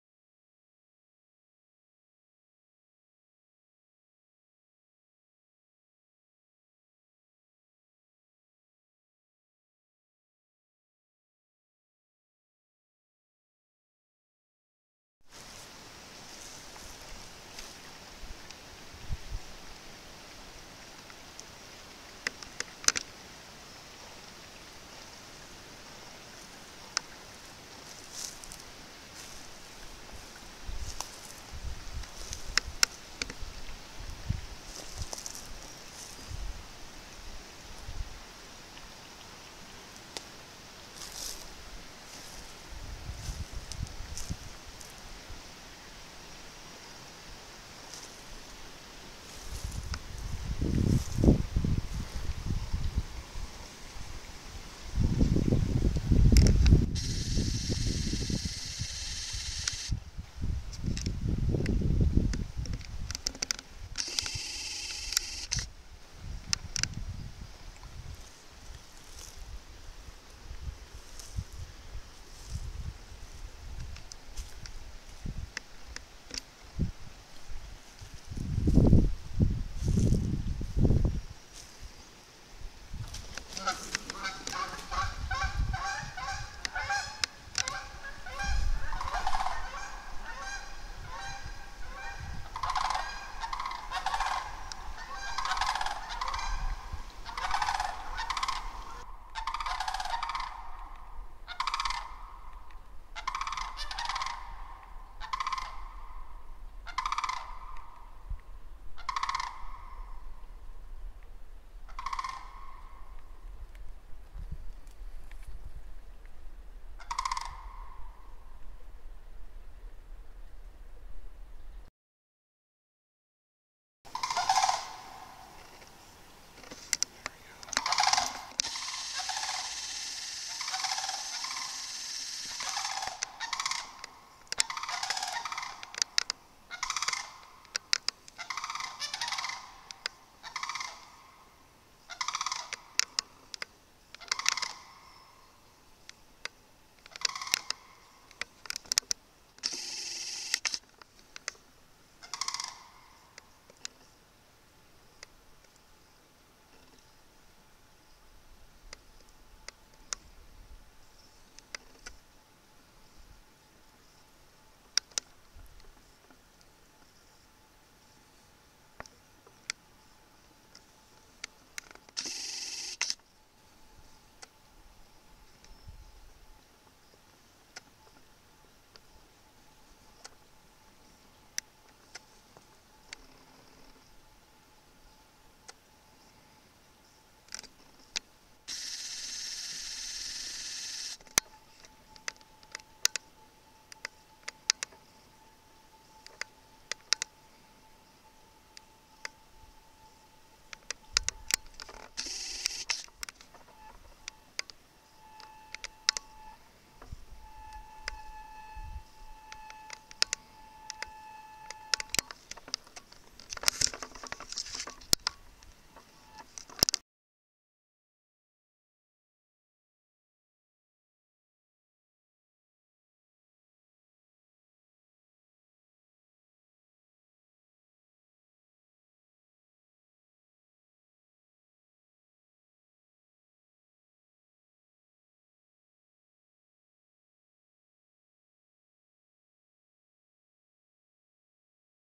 Sandhill Crane and Canadian Geese audio, video and photos of a sitka blacktail deer.
Nature scenery  and audio of Canadian Gees and Sandhill Cranes from along a creek and marshy area in the Tongass forests of SE Alaska.